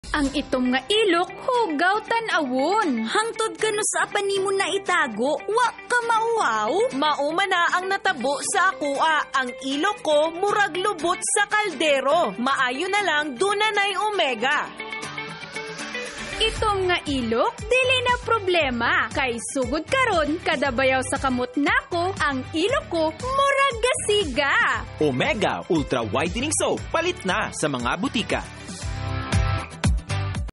Omega Whitening Soap Radio Commercial 2022 30s (in Cebuano)
A Provincial Radio Commercial for their Omega Whitening Soap available only in selected areas and/or provinces of Cebu.

DRPS Two 1295: Omega Whitening Soap Radio Commercial 2022 30s (in Cebuano)